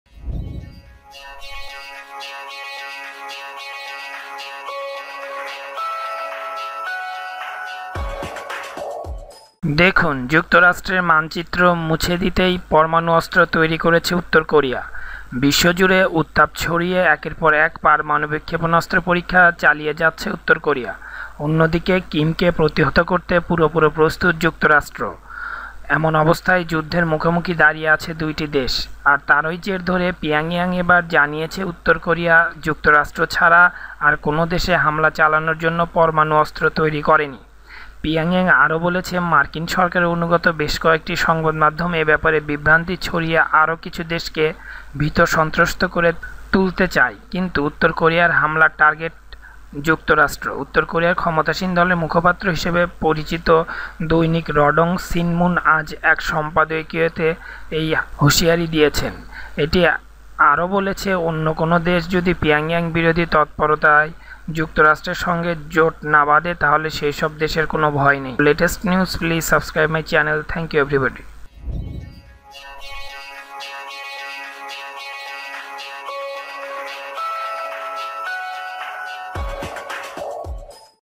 দেখুন যুক্তরাষ্ট্রের মানচিত্র মুছে দিতেই পরমাণু অস্ত্র তৈরি করছে উত্তর কোরিয়া
বিশ্বজুড়ে উত্তাপ ছড়িয়ে একের পর এক পারমাণবিক ক্ষেপণাস্ত্রের পরীক্ষা চালিয়ে যাচ্ছে উত্তর কোরিয়া। অন্যদিকে, কিমকে প্রতিহত করতে পুরোপুরি প্রস্তুত যুক্তরাষ্ট্র।
এমন অবস্থায় যুদ্ধের মুখোমুখি দাঁড়িয়ে আছে দু'টি দেশ। আর তারই জের ধরে পিয়ংইয়ং এবার জানিয়েছে, উত্তর কোরিয়া যুক্তরাষ্ট্র ছাড়া আর কোনো দেশে হামলা চালানোর জন্য পরমাণু অস্ত্র তৈরি করেনি।

পিয়ংইয়ং আরও বলেছে, মার্কিন সরকারের অনুগত বেশ কিছু সংবাদমাধ্যম এই ব্যাপারে বিভ্রান্তি ছড়িয়ে আরও কিছু দেশকে ভীত সন্ত্রস্ত করে তুলতে চায়। কিন্তু উত্তর কোরিয়ার হামলার একমাত্র টার্গেট যুক্তরাষ্ট্র। উত্তর কোরিয়ার ক্ষমতাসীন দলের মুখপত্র হিসেবে পরিচিত দৈনিক রডং সিনমুন আজ এক সম্পাদকীয়তে এই হুঁশিয়ারি দিয়েছে। এটি আরও বলেছে, অন্য কোনো দেশ যদি পিয়ংইয়ং বিরোধী তৎপরতায় যুক্তরাষ্ট্রের সঙ্গে জোট না বাধে তাহলে সে সব দেশের কোনো ভয় নেই।

উত্তর কোরিয়া এর আগে ঘোষণা করেছে, যুক্তরাষ্ট্র বিদ্বেষী নীতি এবং হামলা চালানোর হুমকির কারণে পরমাণু অস্ত্র তৈরি করতে বাধ্য হয়েছে পিয়ংইয়ং। দেশটি আরও বলেছে, যুক্তরাষ্ট্র যাতে পরমাণু যুদ্ধ শুরু করতে না পারে সেজন্য আত্মরক্ষার স্বার্থে এই অস্ত্র তৈরি করেছে দেশটি। এছাড়া, শনিবার উত্তর কোরিয়া বেশ কিছু পোস্টার বিলি করেছে যেখানে দেখা যাচ্ছে উত্তর কোরিয়ার ক্ষেপণাস্ত্র যুক্তরাষ্ট্র আঘাত হানছে।